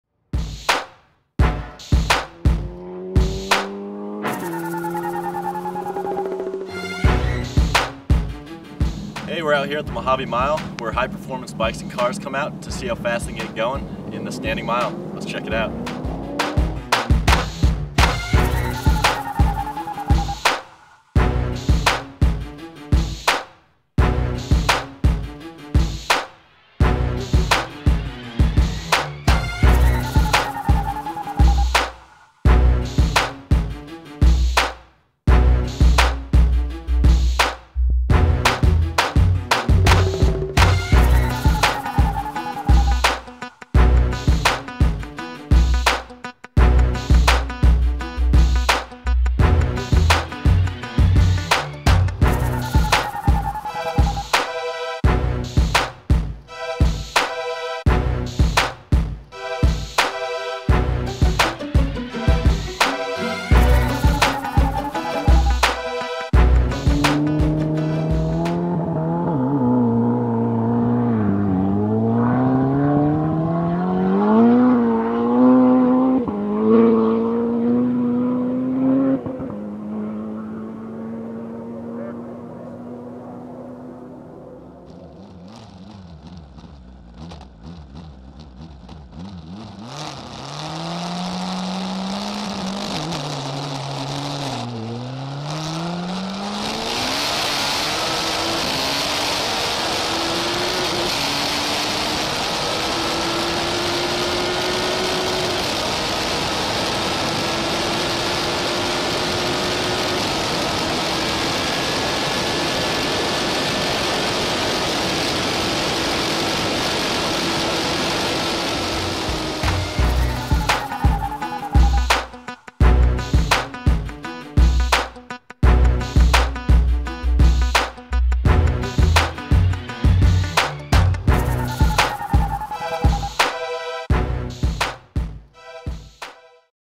Mojave Mile Recap - 208 MPH Turbo Hayabusa Run
Footage from the May 2012 Mojave Mile event. The fastest motorcycle run of the weekend was 208.2 MPH on a 2007 turbo Hayabusa. Desert heat and wind played a role in the overall best standing mile top speeds achieved by all riders/drivers involved. Full gear and tech inspection approval was required before entering onto a hot track.